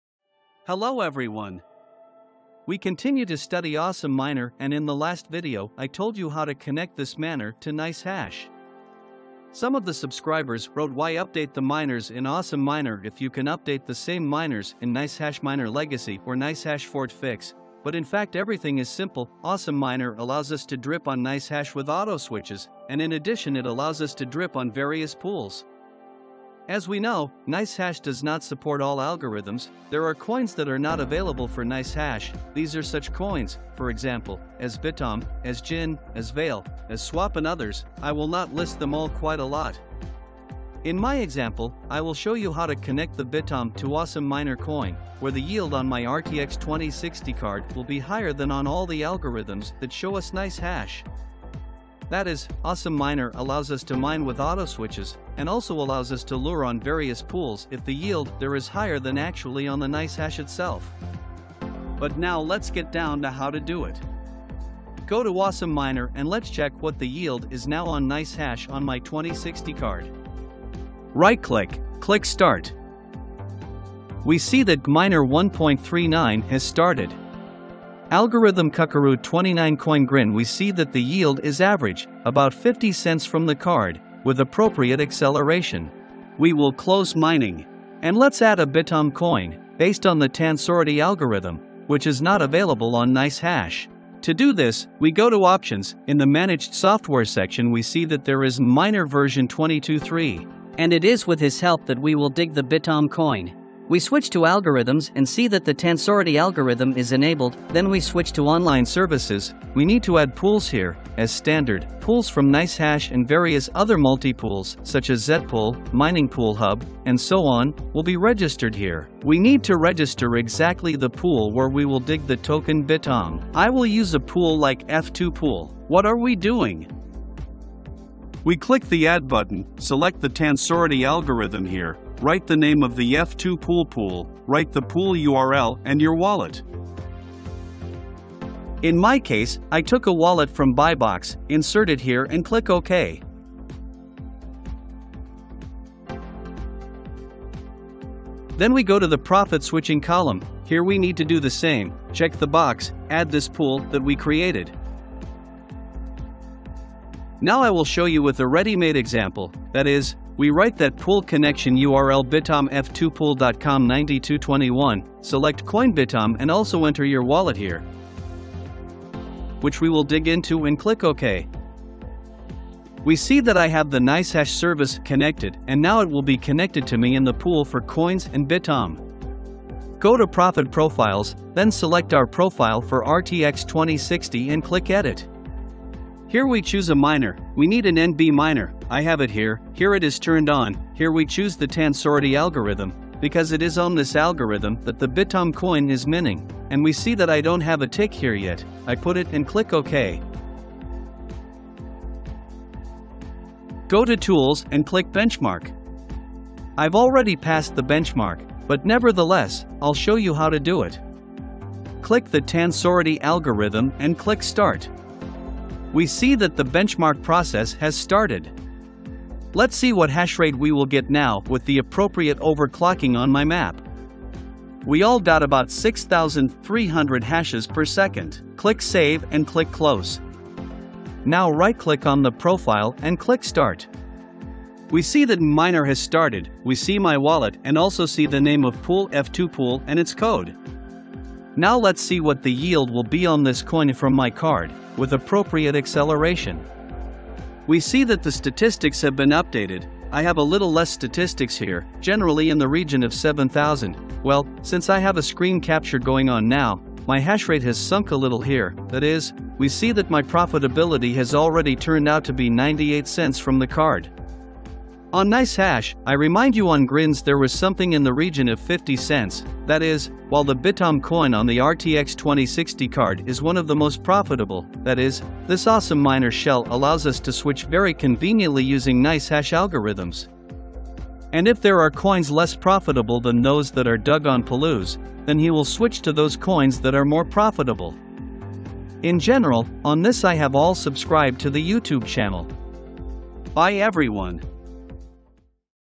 ✔ WHY IS AWESOME MINER BETTER THAN NICEHASH MINER LEGACY? HOW TO CONNECT POOLS TO AWESOME MINER? ✔
In this video I will tell you why in my opinion awesome miner is better than nicehash miner legacy or nicehash fork fix, I will also show you how to connect non-standard pools to awesome miner using the example of f2pool for the bytom coin!